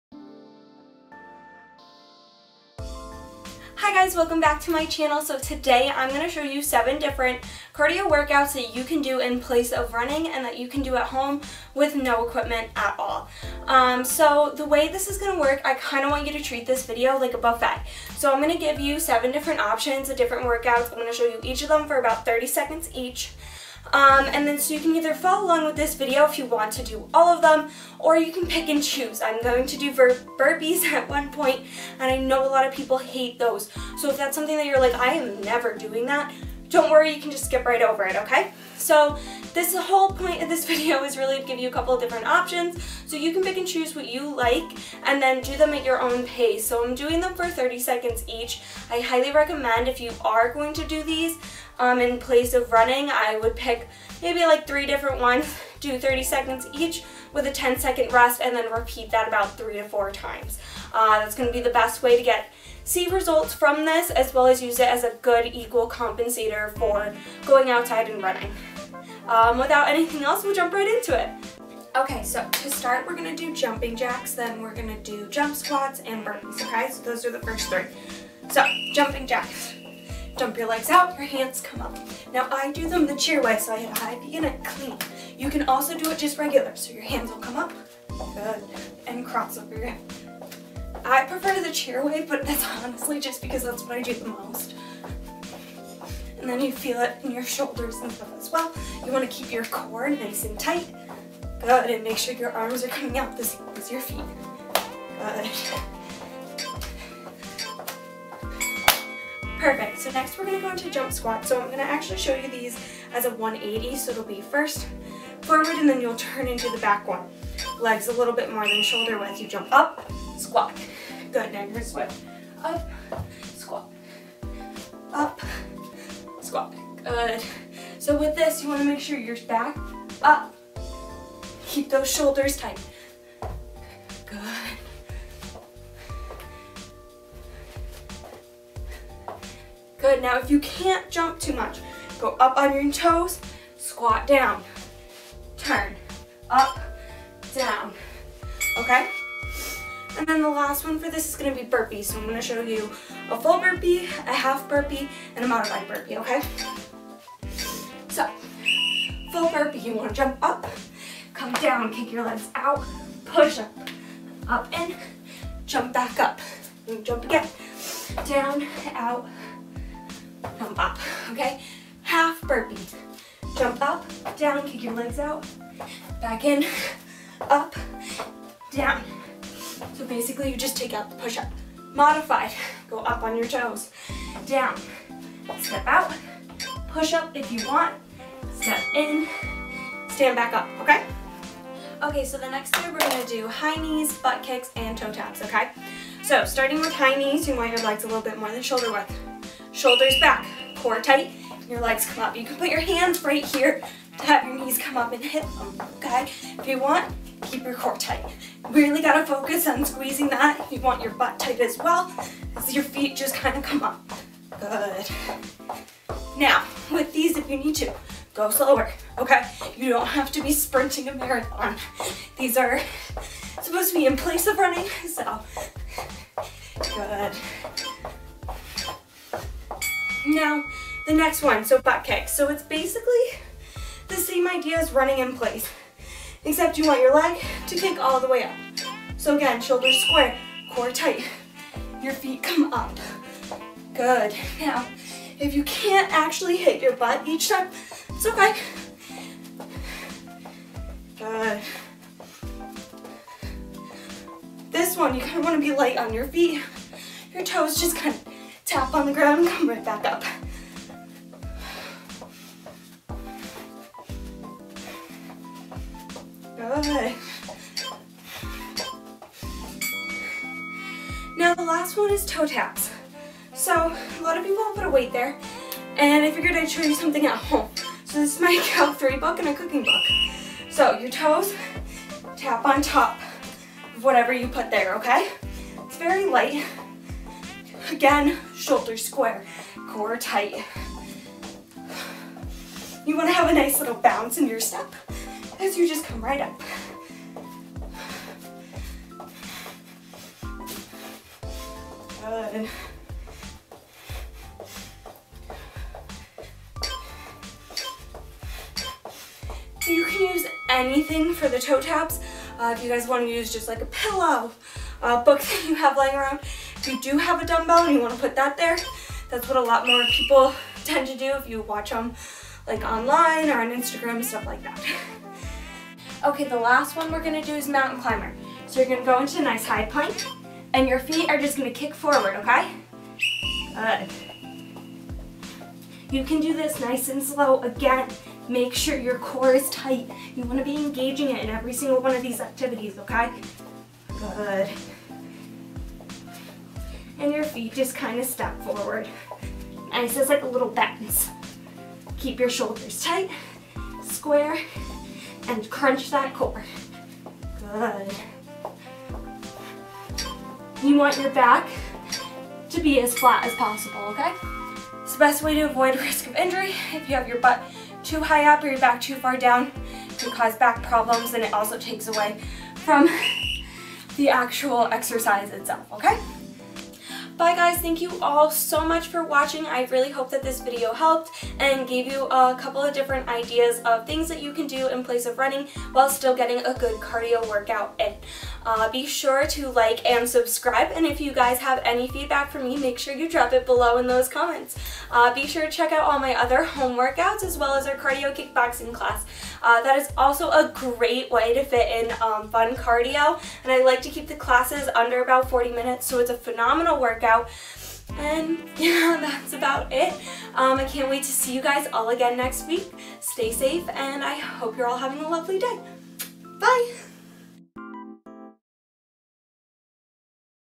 Cardio Alternatives to Running
Hello loves!
I know a lot of people do not enjoy running, I am included in that grouping.
Here are some simple, but EFFECTIVE alternative workouts to do in place of running!
To get the most out of these I would do them in a HIIT fashion, meaning I would do the exercise for about 20-30seconds on with a 10-15second rest. I love to pick 2-3 of these workouts and make a mini circuit as a warm up before my workout.
An example workout including these would be:
Warm-up, 20seconds on with a 10 second rest, repeated 3x
1. Jumping Jacks
2. Burpees
3. Mountain Climbers
Followed by my "At Home Abs Workout!!"

A second example would be:
Warm-up: 30seconds on with a 15second rest, repeated 3x
1. Jump Squats
2. Toe Taps
Followed by my "Booty & Abs Home HIIT Workout!"

I hope these alternative cardio workouts are helpful, along with these example workouts!

I am doing FREE PERSONALIZED workout plans for anyone who is interested!!

I hope you all are staying safe and enjoying the nice weather :)